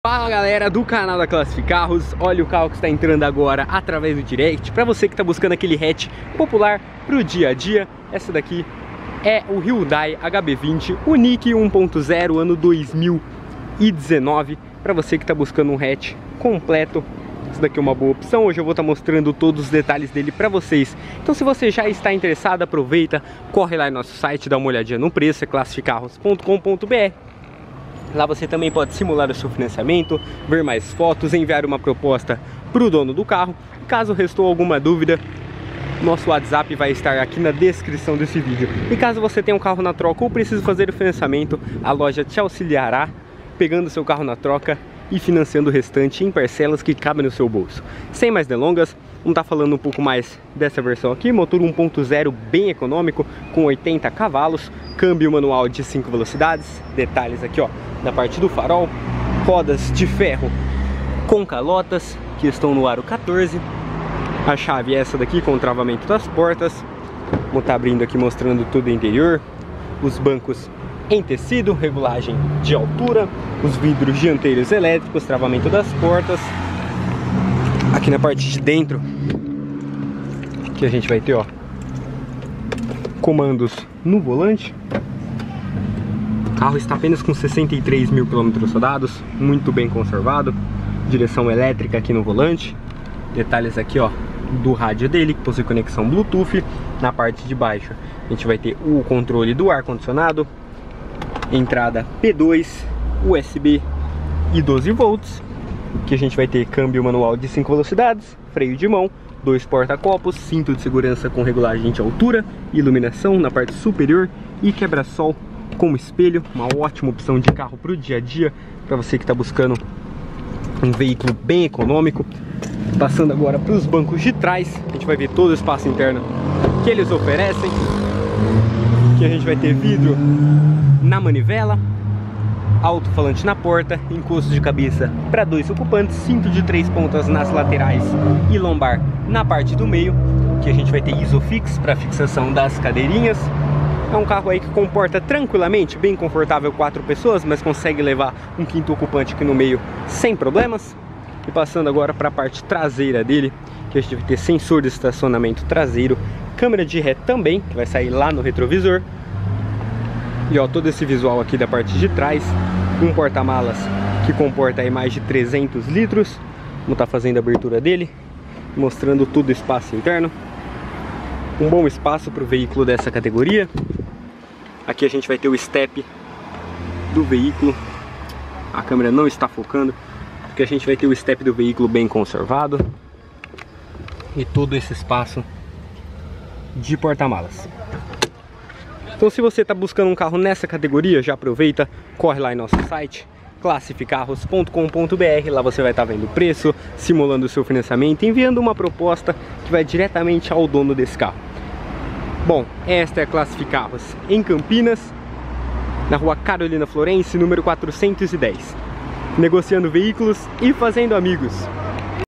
Hyundai HB20 Unique 1.0 (Flex) Manual 2019
Sobre esse carro: IMPECÁVEL (ÚNICA DONA) C/ MANUAL, CHAVE RESERVA, MANUTENÇÕES NA CONCESSIONÁRIA E 1 ANO DE GARANTIA. HATCH COMPLETO PERFEITO PARA O DIA A DIA. C/ MOTOR ECONOMICO FLEX 80CV, CÂMBIO MANUAL, DIREÇÃO ELÉTRICA, RÁDIO C/ BLUETOOTH, PORTA MALAS DE 300L